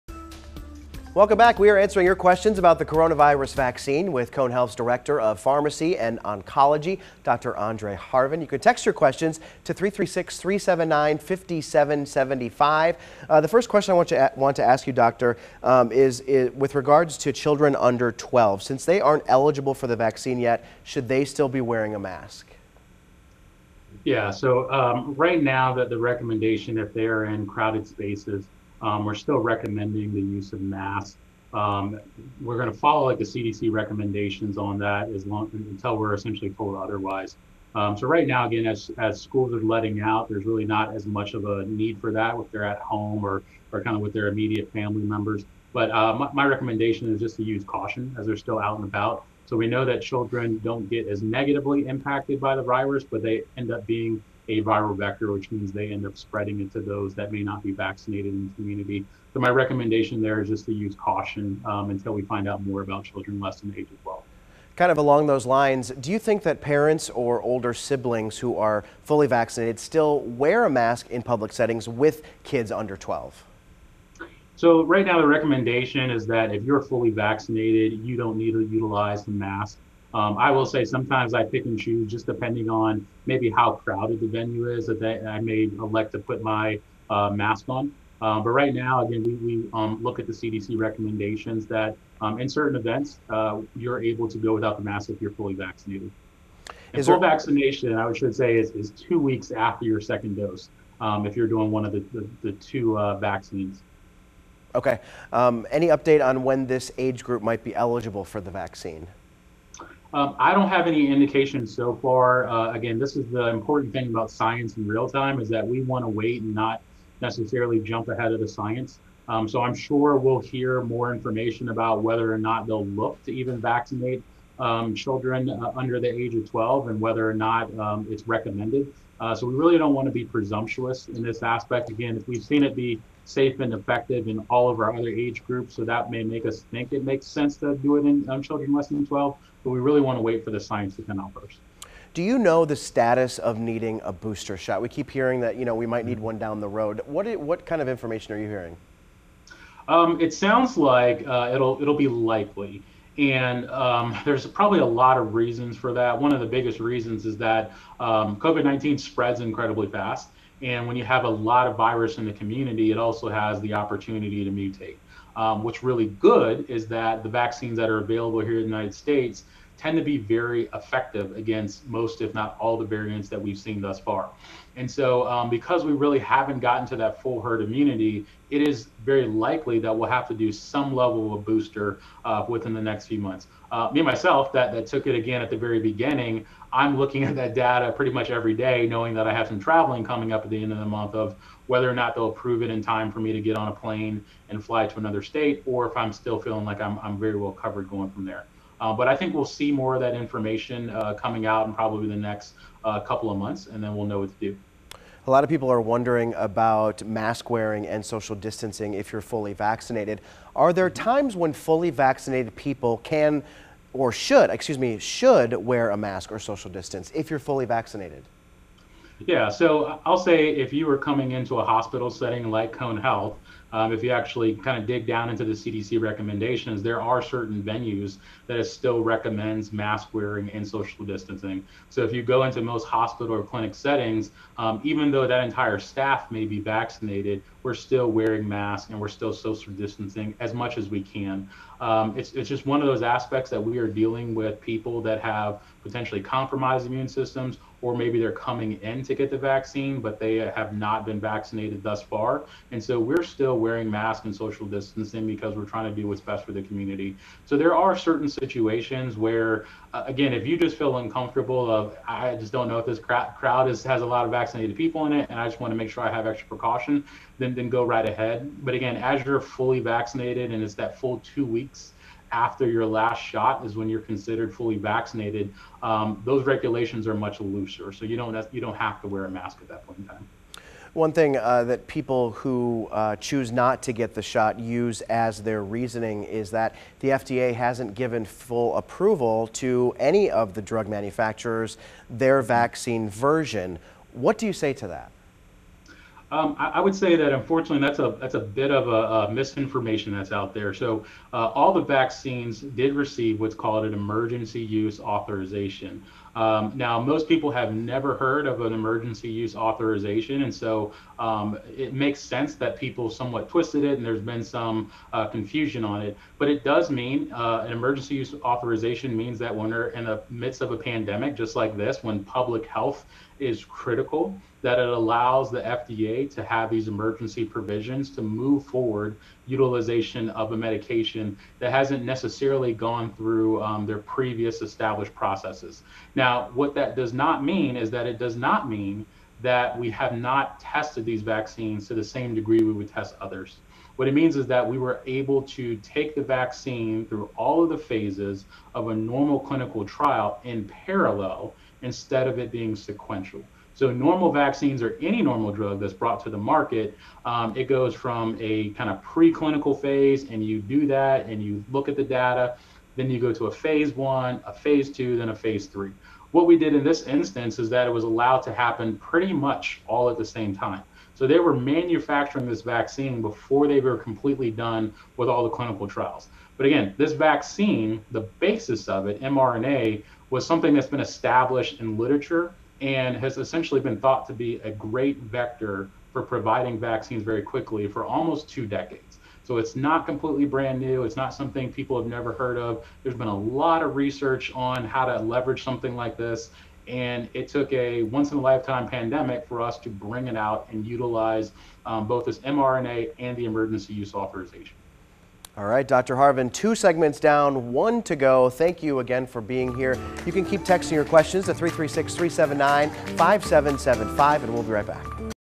Cone Health answers your coronavirus vaccine questions: Part 1
A Cone Health expert answers your coronavirus vaccine questions.